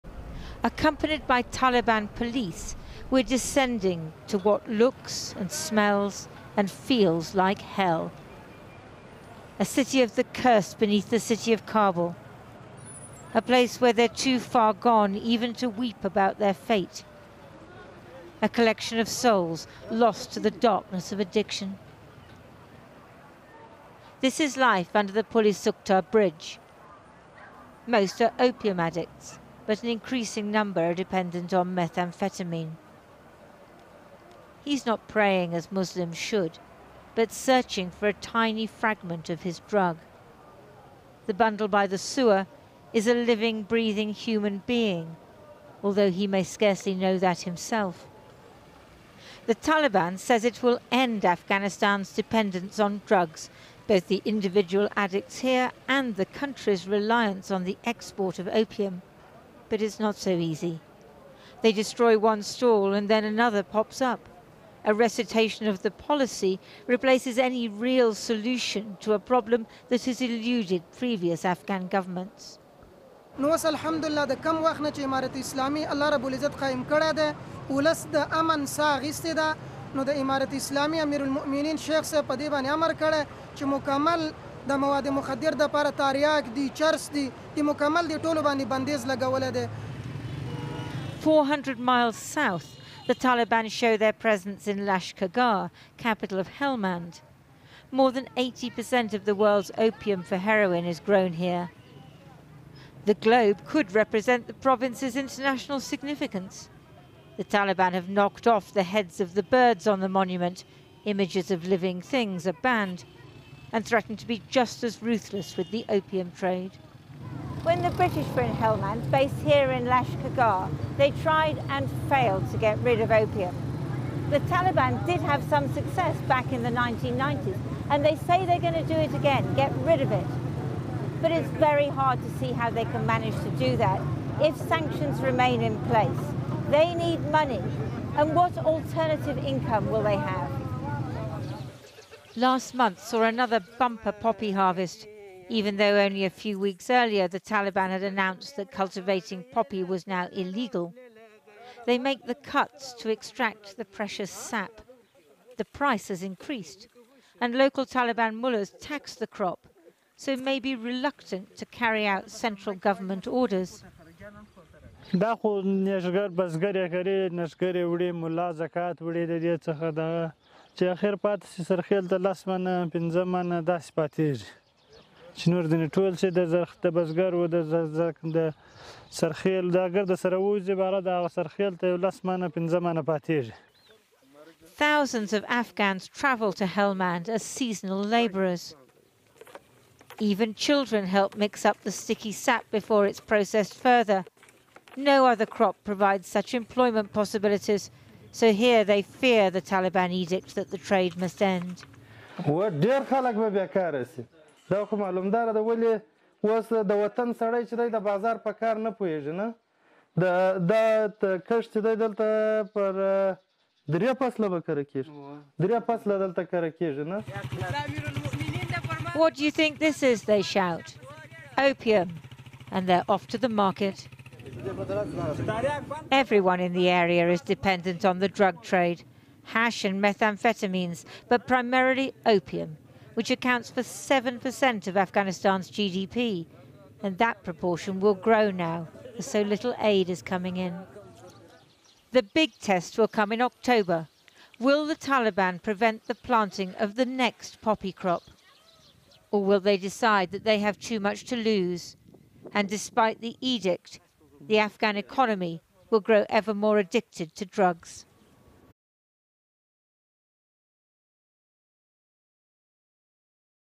Taliban's drug trade problem: the reality of Afghanistan's opium addiction
An edict by Afghanistan's hardline Taliban rulers to ban poppy cultivation has gone largely ignored.

It's the world leader in producing opium and heroin and with aid now stopped because of sanctions, enforcing the ban would impoverish the country even further - and could erode support for the Taliban at the same time.

The bulk of the country's opium is grown south of Kabul in the Taliban heartland of Helmand province.

Producer: Hila May
Camera/ Edit: Soren Munk.